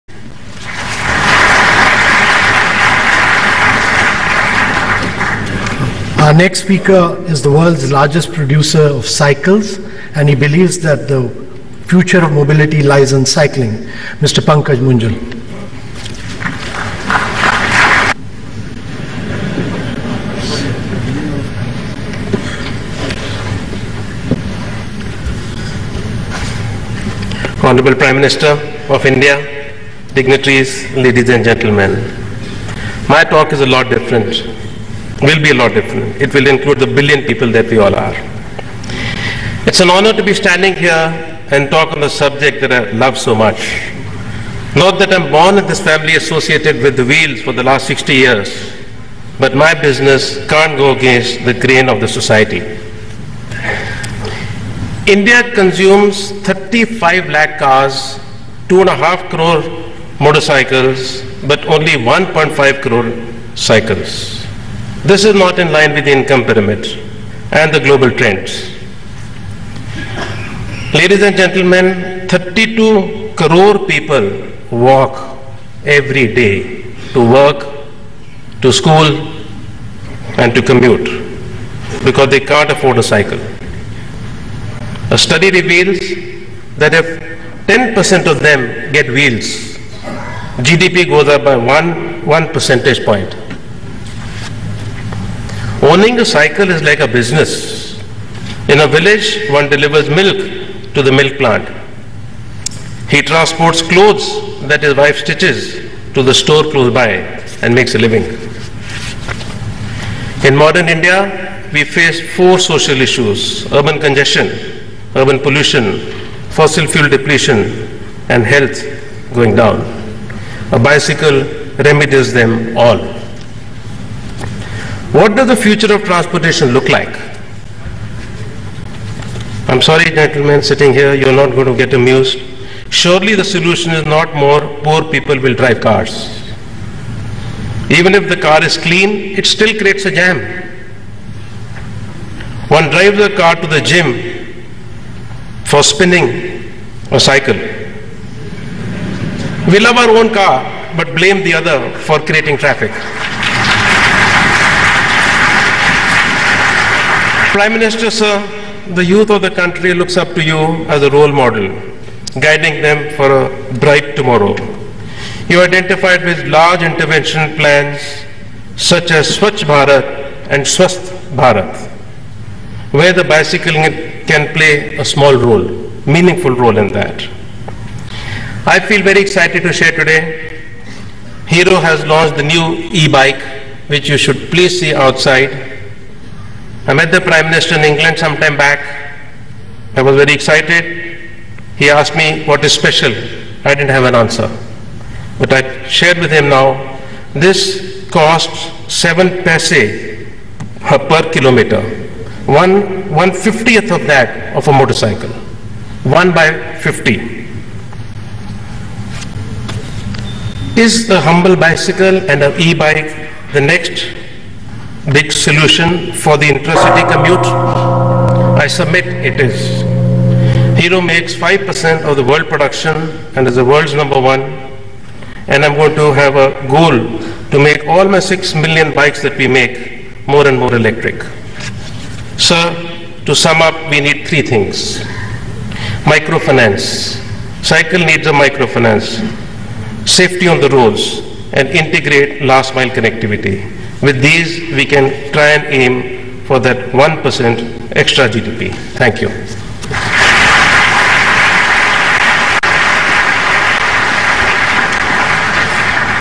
Address of Mr Pankaj M Munjal - MOVE Summit organised by NITI Aayog
Mr Pankaj M Munjal addressing to the PM of India along with other dignitaries present at India's first mobility Summit - MOVE, organised by NITI Aayog at Delhi.